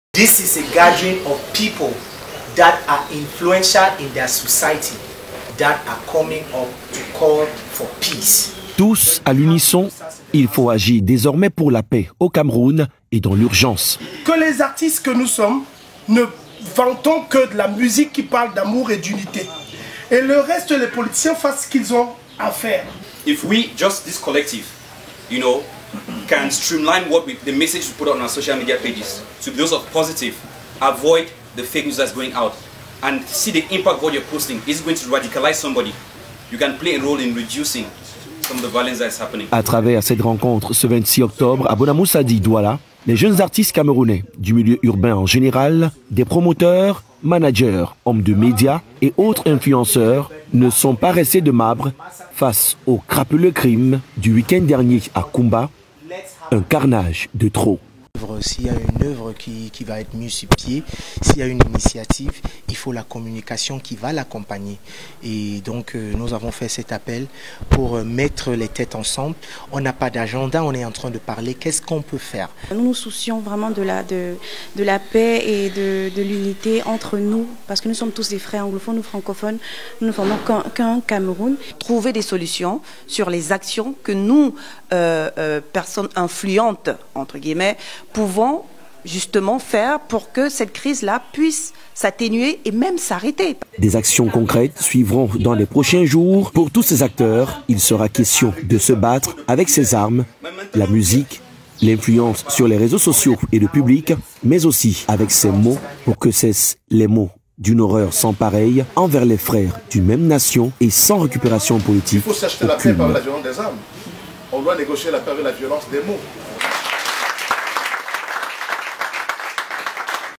Salatiel, Mr. Leo, Daphne, Magasco & more Kamer artist held a meeting to restore Peace in Cameroon
What Significance will the Cry of Cameroonian celebrities play in resolving the Anglophone Crisis
Legal practitioners and political analysts, discuss facts about the Kumba Massacre
Where were the teachers, gate-men or school security during the Kumba Massacre?
No Comment CMTV,
No Comment TV

Kamer artists held a meeting to restore peace

CMTV Cameroon
CMTV Buzz